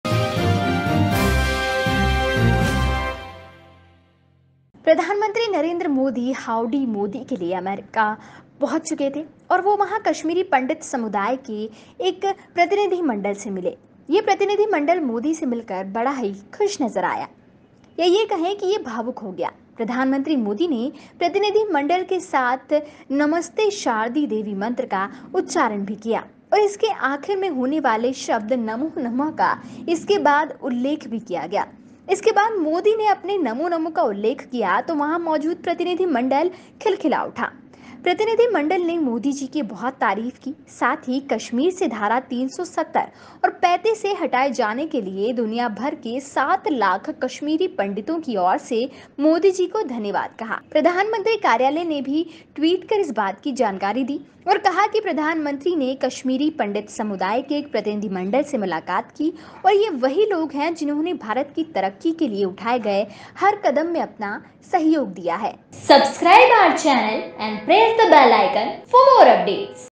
Modi से मिलकर कश्मीरी पंडित क्यों रो दिए | PM Modi Meets Kashmiri Pandit | HowdyModi | Ghamasan
धारा 370 हटाए जाने से खुश हैं कश्मीरी पंडित |